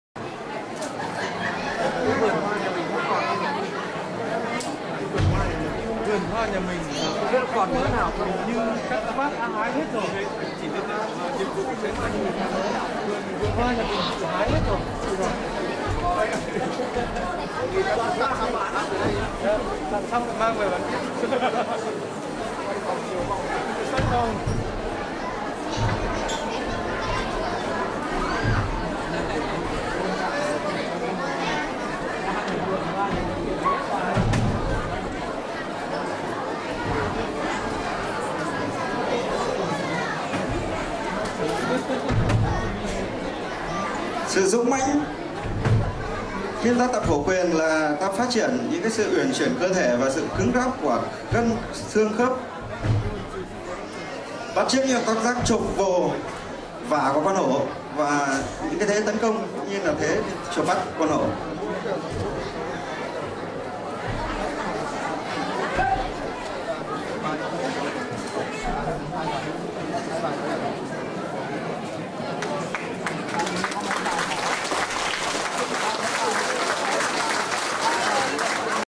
[武氣道] Võ Khí Đạo | Bạch Hổ Quyền - Tigerform
Vo Khi Dao präsentiert eine Tierform auf der Feier zum vietnamesischen Neujahr in Nürnberg 2011.